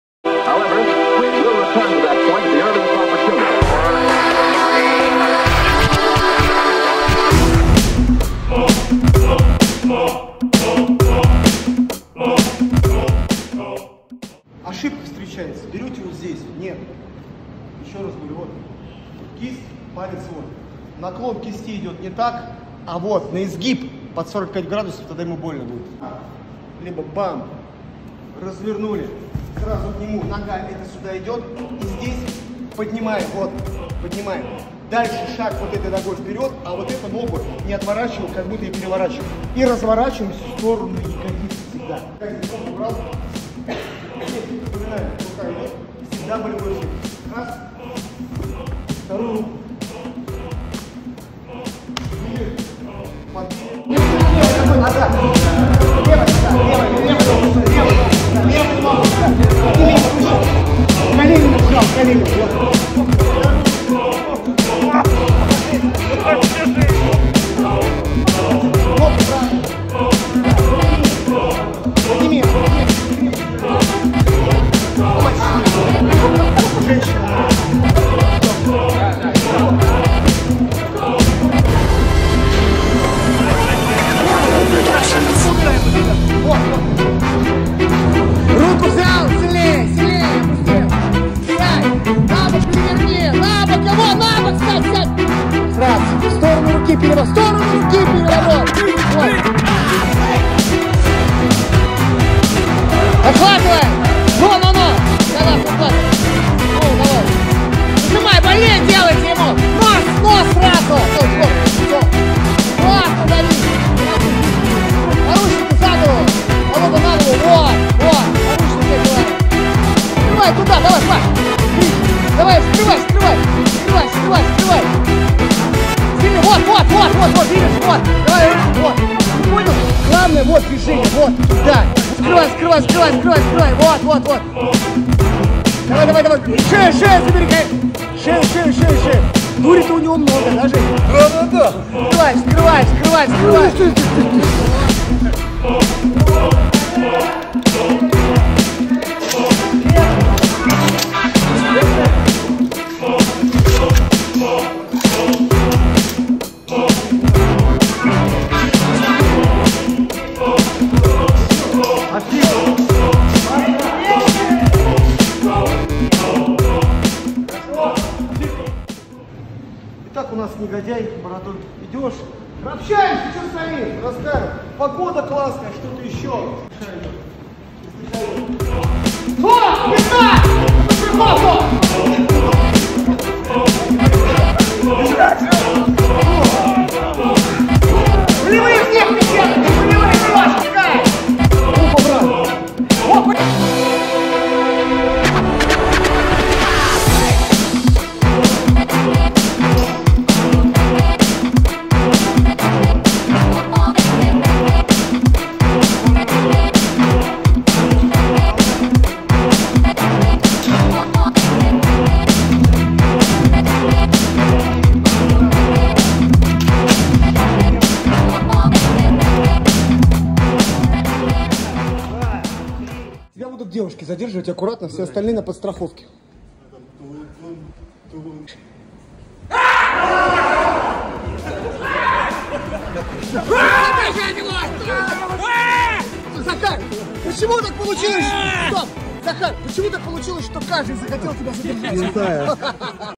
ЭЛЕМЕНТЫ ЗАДЕРЖАНИЯ и ЭМИТАЦИЯ ТАКТИКИ / клуб ЯРОПОЛК / 151 урок ДОВОЕННОЙ ПОДГОТОВКИ, 11.05.2024
11 мая 2024 г. прошёл 151 урок довоенной подготовки в СПК «ЯРОПОЛК».

На уроке были отработаны следующие вопросы:
✔️Элементы задержания ,
✔️Имитация тактики в городской застройке

Наши уроки бесплатные.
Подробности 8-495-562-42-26
3. Мой канал в РУТУБ